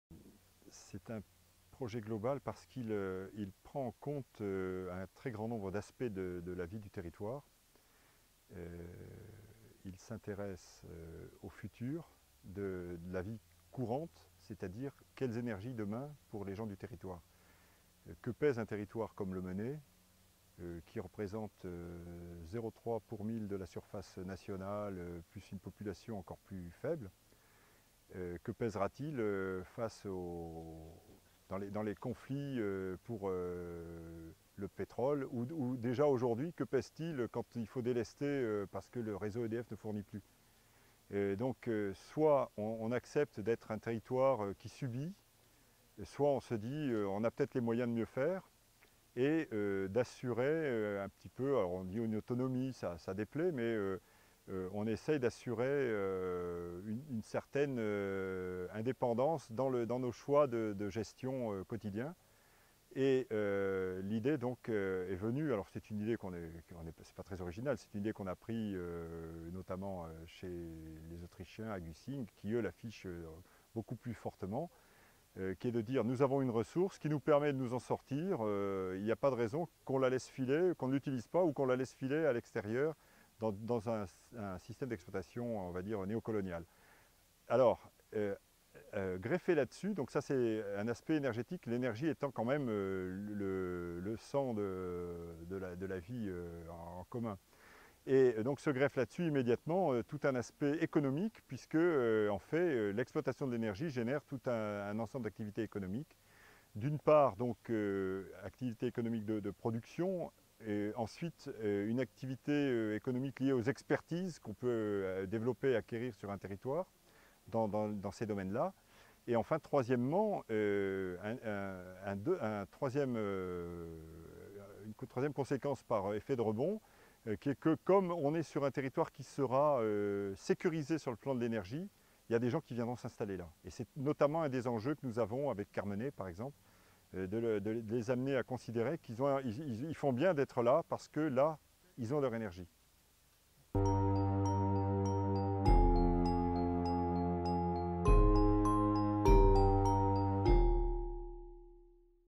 Interview de Marc Théry, chargé de mission énergie de la communauté de communes du Mené (1)
Question: "En quoi la stratégie énergétique du Mené représente-t-elle un projet de territoire global?"
Interview de Marc Théry, chargé de mission énergie de la communauté de communes du Mené, lors du voyage d'études "vers des territoires à énergie positive" organisé par le CLER en centre Bretagne les 11-12-13 octobre 2010